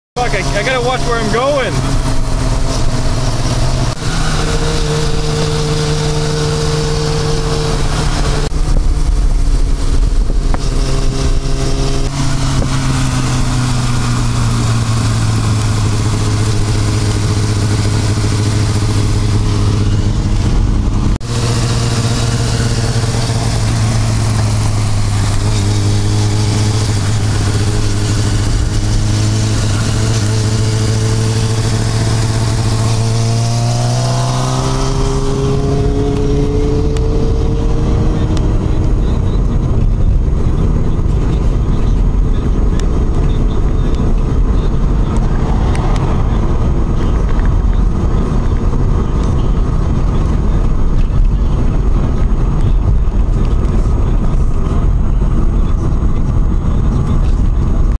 Aug 03'07 - Night Riding *giggle*
Finally a video that features me on my motorcycle in ACTION!

No more first person view, my visual perception, helmet cam, etc. but instead a third person capture of me riding my bike at night.

Nothing really spectacular, camera operation needs a lotta work, but at least you get to see me ride around!

I know, I have no leathers or any kinda gear on... SQUID... but it was a hot night and I didn't go far... just to the local Mac's to get a slushie and chill.